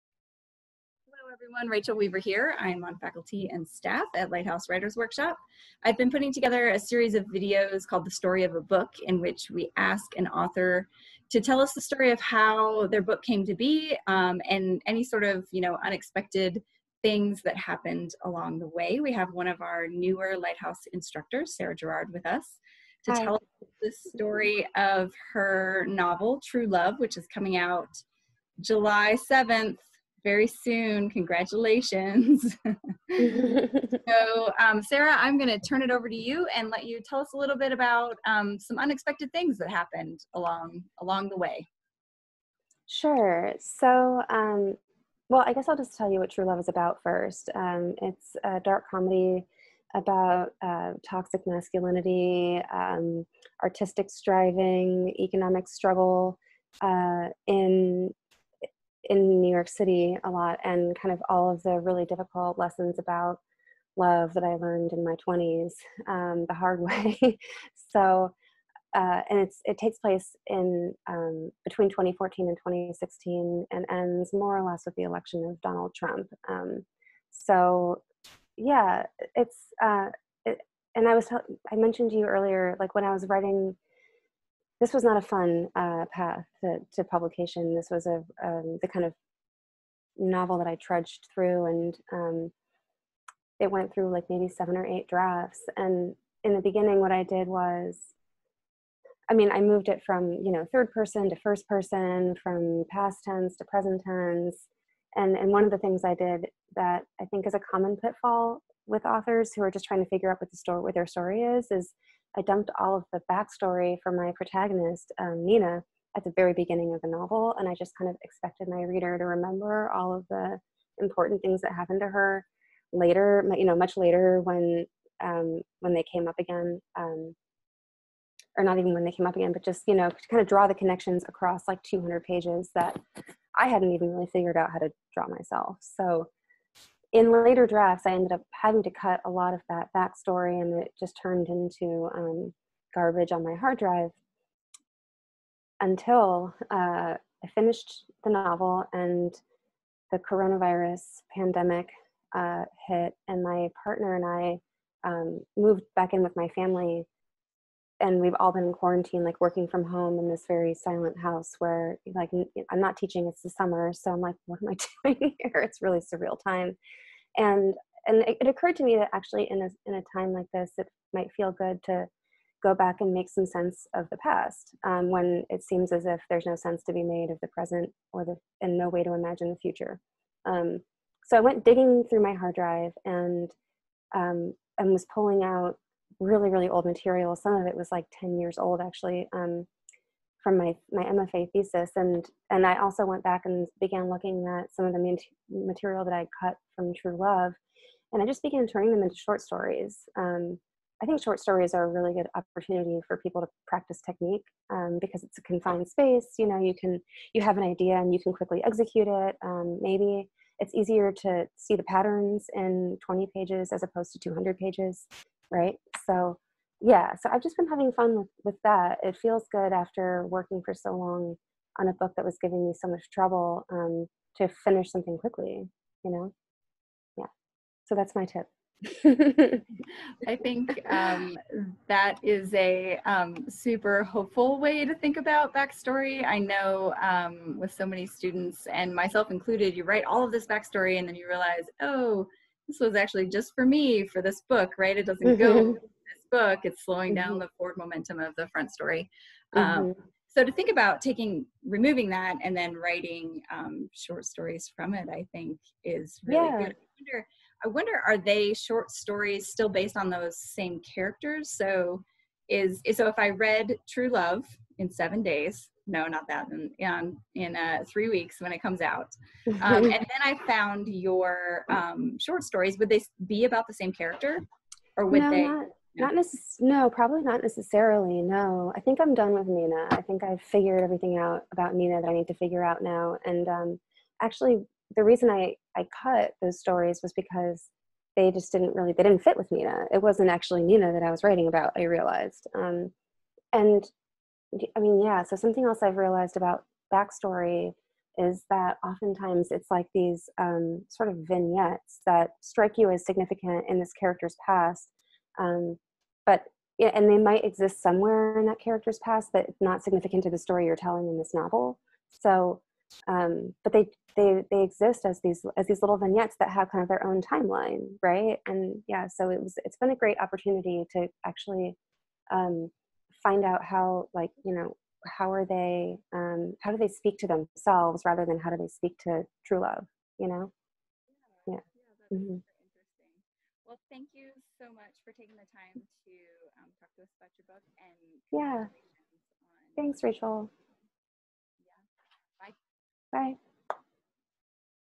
The Story of a Book with Sarah Gerard
In this series of videos, Lighthouse instructor Rachel Weaver asks authors to tell us the story of how their book came to be in the world. In this video, Lighthouse instructor Sarah Gerard talks about the troublesome backstory in her new novel, True Love.